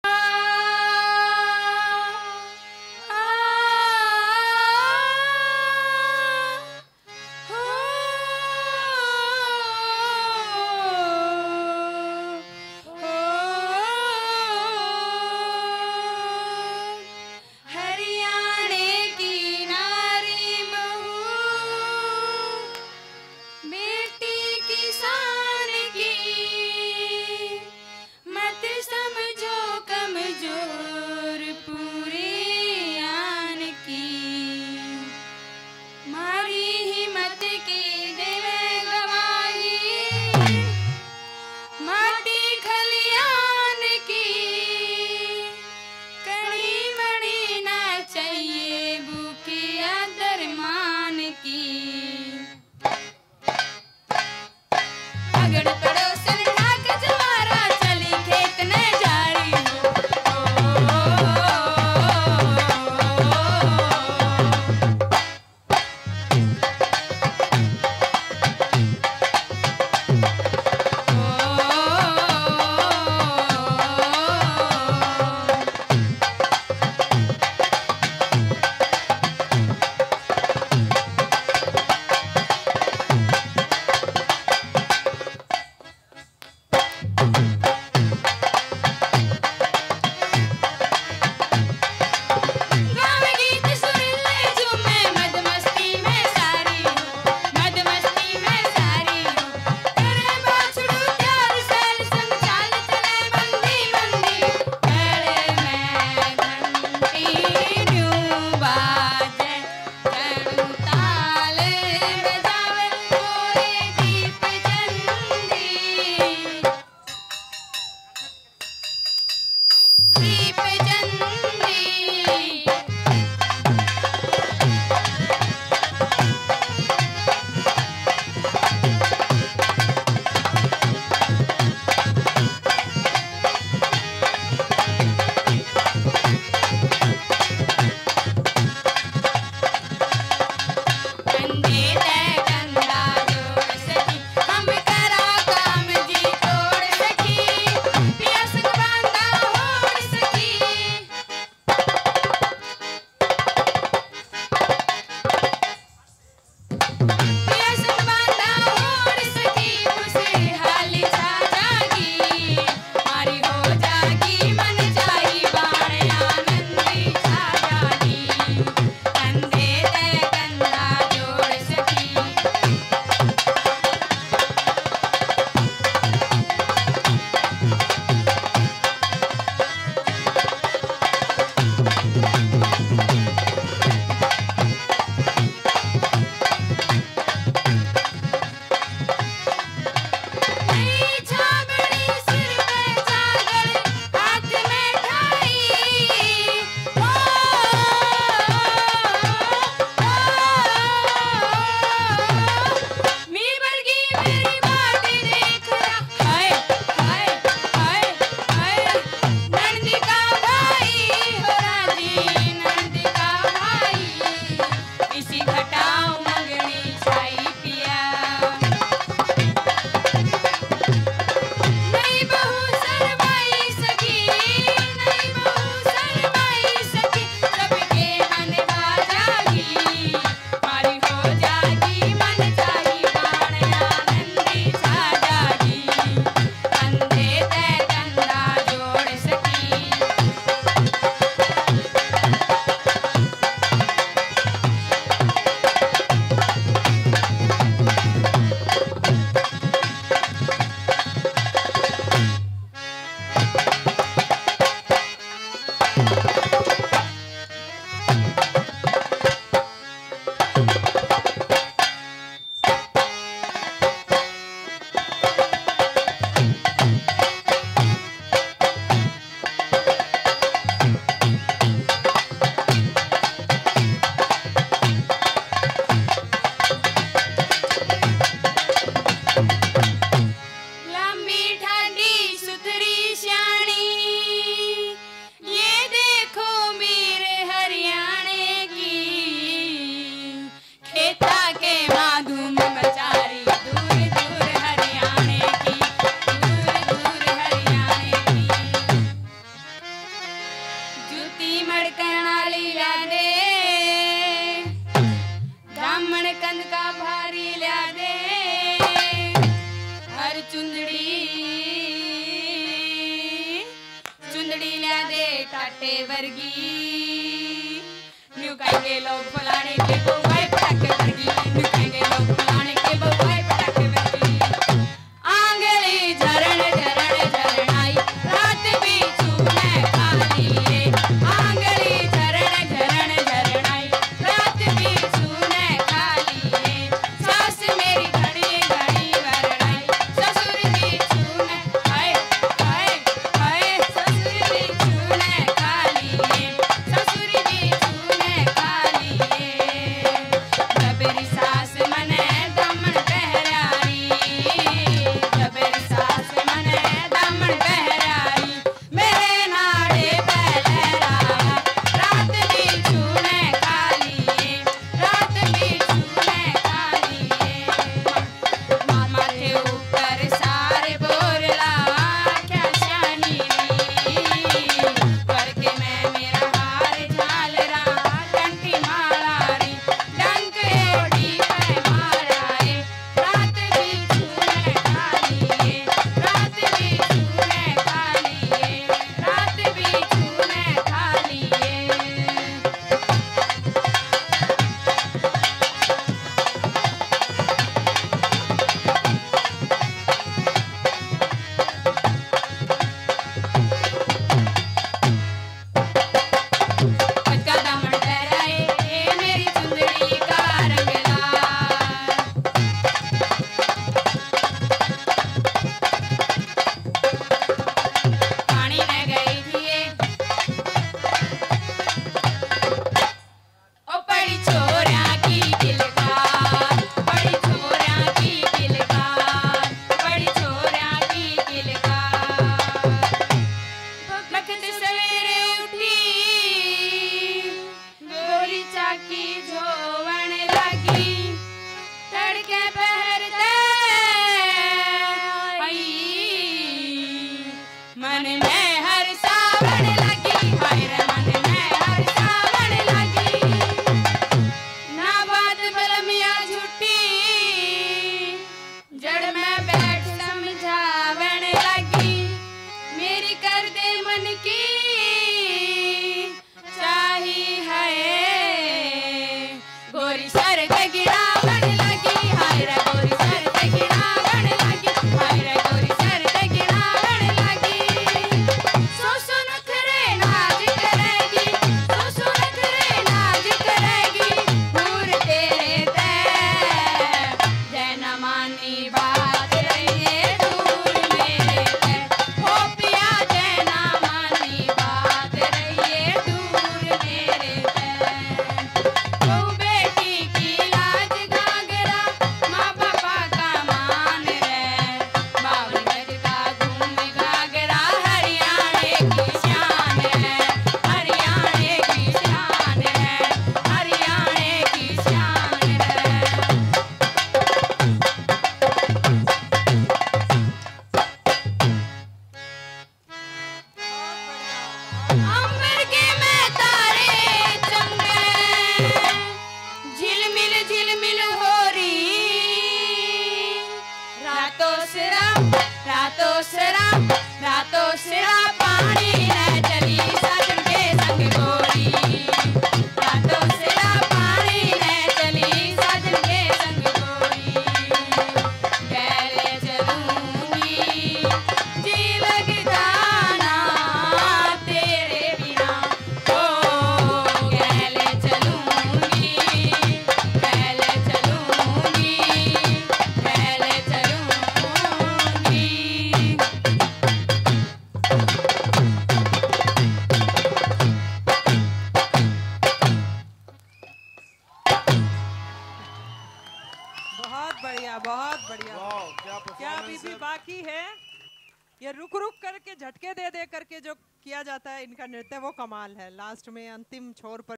महारानी लक्ष्मीबाई कॉलेज के यूथ फेस्टिवल में गज़ब हरियाणवी डांस
साथ ठुमके लगाने को जी करेगा आपका